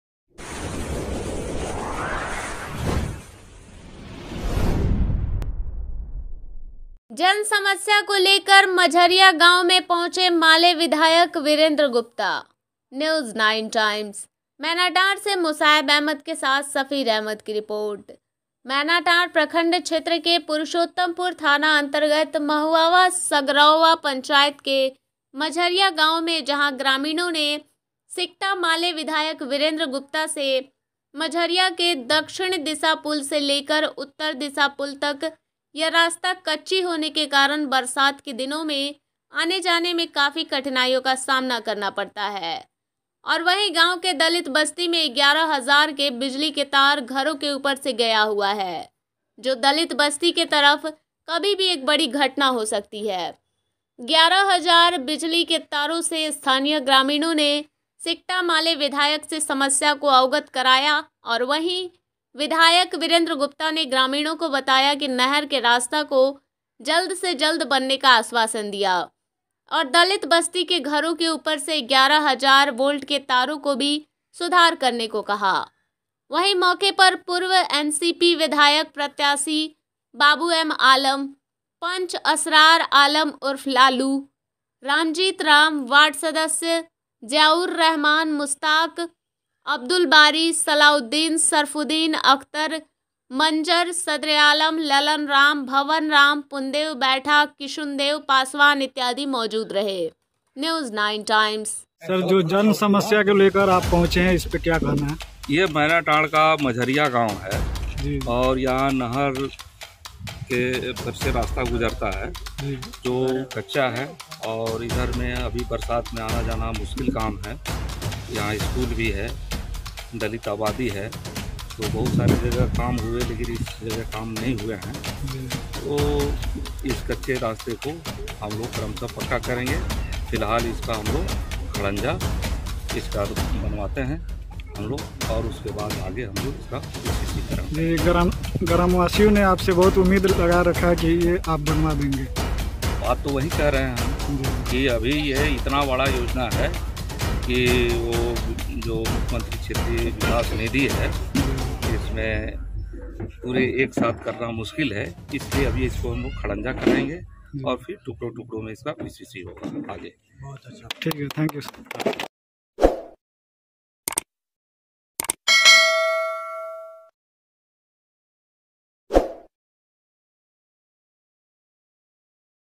जन समस्या को लेकर मंझरिया गांव में पहुंचे माले विधायक वीरेंद्र गुप्ता।
न्यूज़9टाईम्स:- मैनाटांड़ से मोसाहेब अहमद के साथ सफीर अहमद रिपोर्ट।

मैनाटांड़ प्रखण्ड क्षेत्र के पुरुषोत्तमपुर थाना अंतर्गत महुअवा सगरौवा पंचायत के मंझरिया गांव में जहां ग्रामीणों ने सिकटा माले विधायक वीरेंद्र गुप्ता से मंझरिया के दक्षिण दिशा पुल से लेकर उत्तर दिशा पुल तक यह रास्ता कच्ची होने के कारण बरसात के दिनों में आने जाने में काफी कठिनाइयों का सामना करना पड़ता हैं, और वही गांव के दलित बस्ती में 11 हजार के बिजली की तार घरों के ऊपर से गया हुआ है जो दलित बस्ती के तरफ कभी भी एक बड़ी घटना हो सकती हैं 11 हजार बिजली के तारों से स्थानीय ग्रामीणों ने सिकटा माले विधायक से समस्या को अवगत कराया वही विधायक वीरेंद्र गुप्ता ने ग्रामीणों को बताया की नहर के रास्ता को जल्द से ज्याद बनने को आश्वासन दिया और दलित बस्ती के घरों के ऊपर से 11 हजार तारों को भी सुधार करने को कहा।वही मौके पर पूर्व NCP विधायक प्रत्यासी बाबू एम आलम,पंच असरार आलम उर्फ लालू,रामजीत राम,वार्ड सदस्य ज्याऊररहमान,मुस्ताक,अब्दुल बारी,सलाउद्दीन,सरफुद्दीन,अख्तर,मंजर,सदरेयालम,ललन राम,भवन राम,पुनदेव बैठा,किशुनदेव पासवान इत्यादि मौजूद रहे।। *news9times